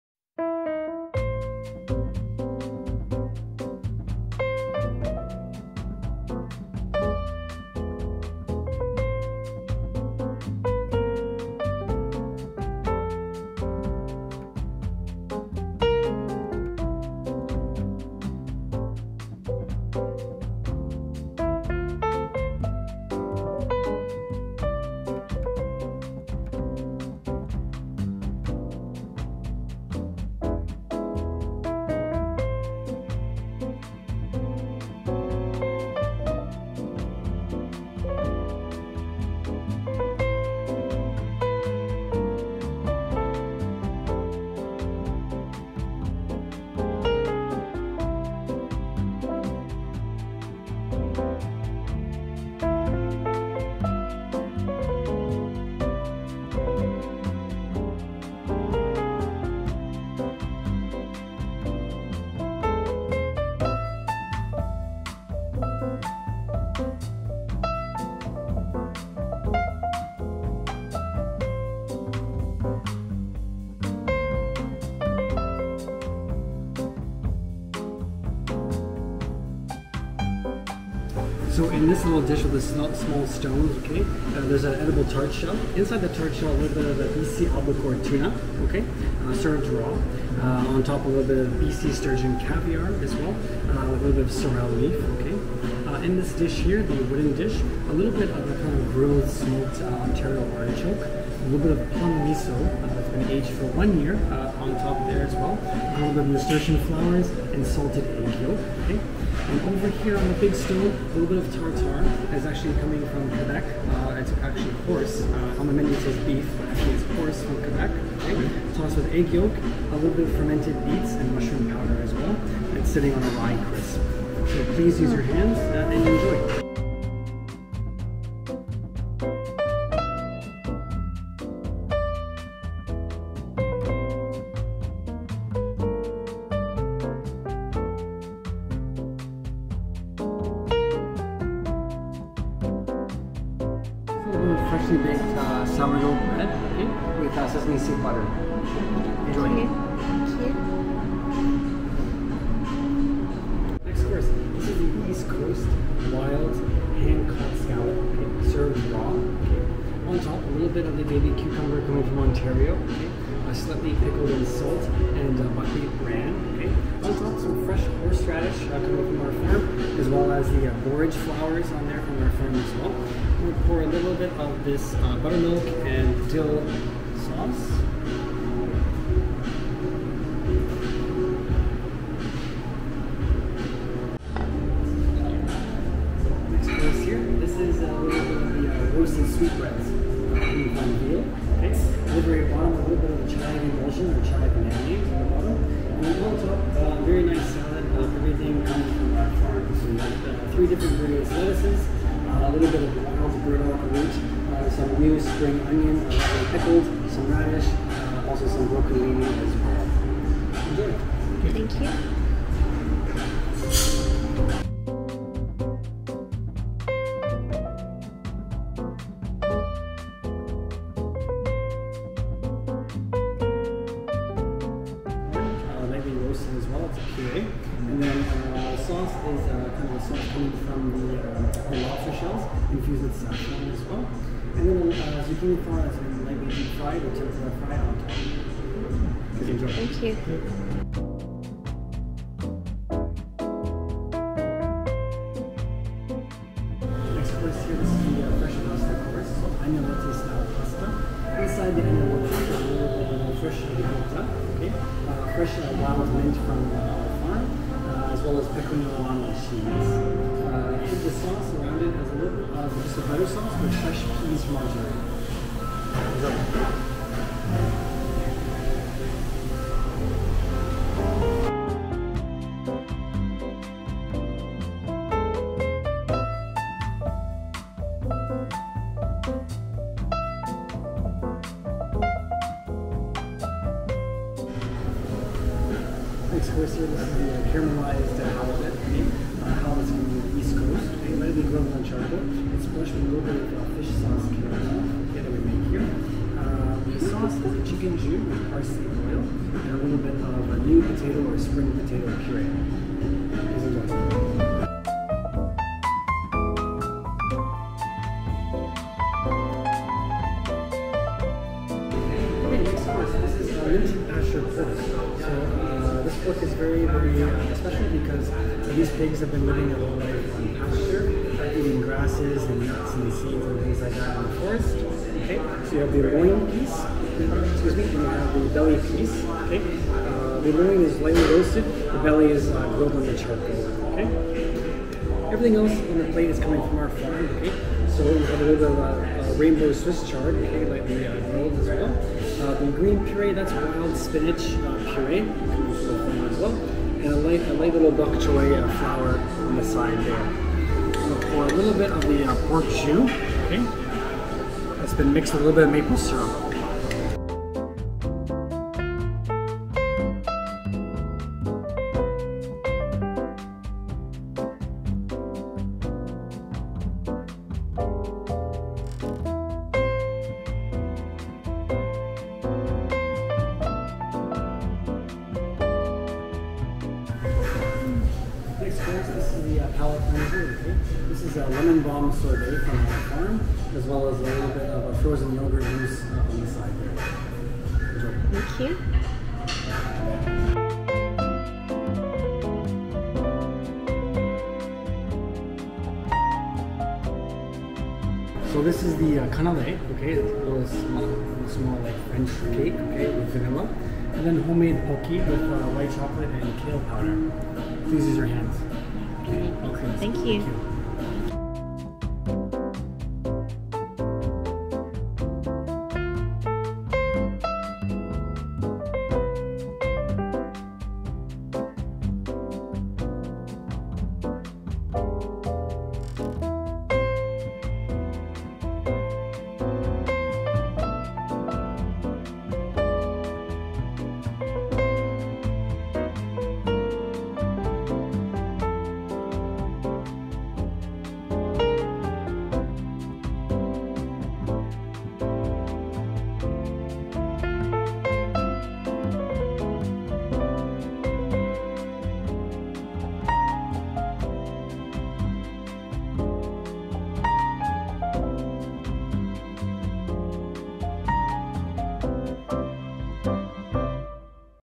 FRILU - MICHELIN STAR
We experience an incredible tasting menu dinner at Frilu Contemporary Fine Dining in Thornhill, Ontario. Just north of the city of Toronto. As I enter Frilu, I immediately feel the welcoming warmth and calm of the restaurant.  Every dish presented to me was an absolute delight, pure joy!!! Everything was amazing!!!  I hope you enjoy the video as it will give you insight into our spectacular experience!  Frilu is a very special restaurant that is a must try!!!

First Course (Snack):
1:23 Albacore Tuna Tartar with Caviar
1:41 Grilled Artichoke with Plum Miso
1:52 Horse Meat Tartare with Fermented Beet over Homemade cracker

Second Course (Intermediary):
2:27 Sourdough Bread with Sesame Seed Butter

Third Course (Intermediary):
2:35 Wild Scallop Sashimi with Baby Cucumber and Dill Sauce

Fourth Course (Intermediary):
3:05 Roasted Sweetbread with Willowolf Farm Harvest (Fresh Greens)

Fifth Course (Intermediary):
3:47 Zucchini Flower Tempura, Lobster Claw with Roasted Corn Puree

Sixth Course (Intermediary):
4:07 Ricotta Pasta with Wild Mint and Fresh Peas in Butter Sauce

Seventh Course (Main):
4:45 Charcoal Broiled Halibut in Chicken Jus

Eighth Course (Main):
5:12 Linton Pasture Pork Loin and Belly with Vegetables

Ninth Course (Palate Cleanser):
6:20 Lemon Balm Sherbert with Licorice Frozen Yogurt

Tenth Course (Desert):
6:33 Vanilla, Dried Kombu, Fermented Strawberry Ice Cream

Eleventh Course (Desert):
6:41 Canelé and Home Made Pocky

We highly recommend this restaurant! And it was a pleasure meeting both head-chefs.

JOHN-VINCENT TROIANO
John-Vincent was raised in a traditional Italian family who centered themselves around the joys of food and company. Ambitiously at the age of twelve, he had already made up his mind to go to culinary school. During his youth, he discovered the wonders of Japanese culture. Marking the beginning of his career, he joined Japanese master chef Hashimoto, learning the true insight of the spirit of selfless hospitality, and the intricacies of Kaiseki cuisine. Adding to his resume were internships at world-famous restaurants like the iconic Noma in Copenhagen, and San Francisco’s 3-Michelin Star Benu. Feeling inspired, it was during these experiences that his Asian repertoire of ideas seemed to comingle perfectly with his love and appreciation for nature and ingredients in its purest form.

Returning to Toronto, he became a Chef de Cuisine at Toronto’s Tuscan restaurant, Tutti Matti, where he continued to draw inspirations from his heritage. Eventually, it was finally time for his own restaurant. FRILU is where he would showcase the simplicities of nature using local and seasonal ingredients, combined with the complexity and intricacy of flavors that are brought by his Asian influences.

SO SAKATA
So Sakata was born in Wakayama, Japan into a family history of working in hospitality. Initially working in traditional Japanese restaurants, he made his way to Canada in pursuit of broadening his career horizons. So dove into the wonders of Italian food. He met Chef Troiano and they both instantly connected over their love for Japanese cuisine and hospitality. So is perfect for FRILU as he brings a wealth of knowledge in Japanese culture to the restaurant that complement the FRILU vision perfectly.